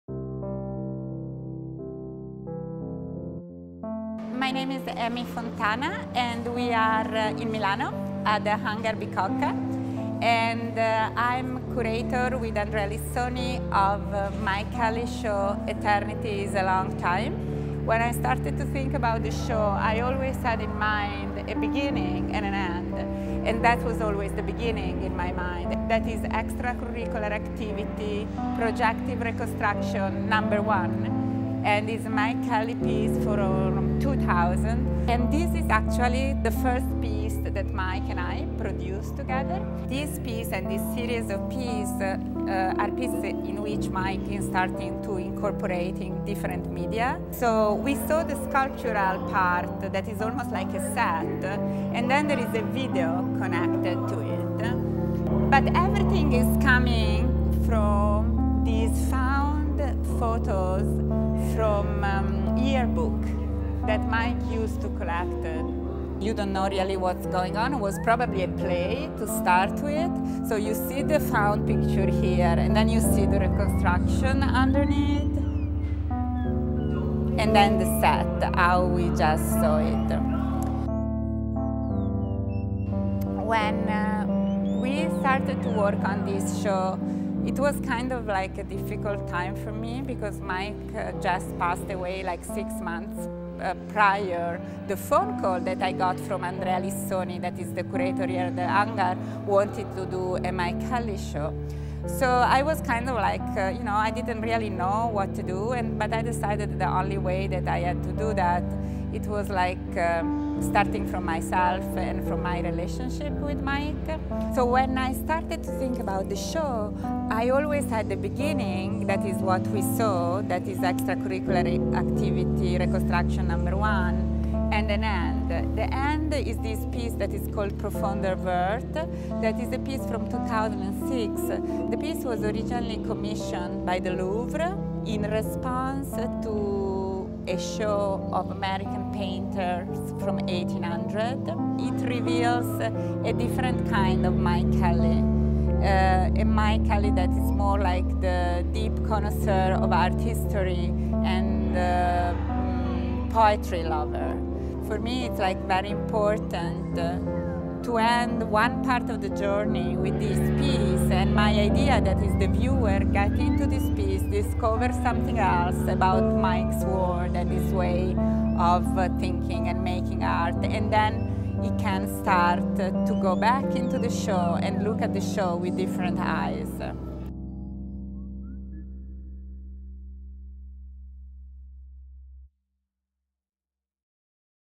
Mike Kelley at Milan's HangarBicocca
Housed in a cavernous warehouse just outside of Milan, the exhibition "Mike Kelley: Eternity is a Long Time," provides visitors with a unique, indulgent experience, exploring the American artist's later work.